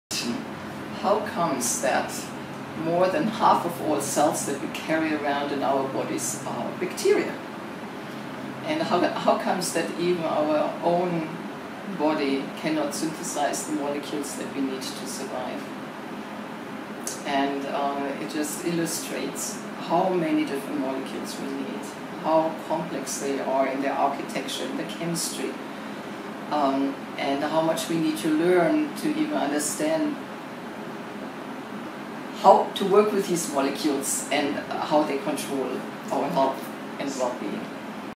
Prof Viola Vogel on Molecular Systems Engineering
Prof.  Viola Vogel explains Molecular Systems Engineering.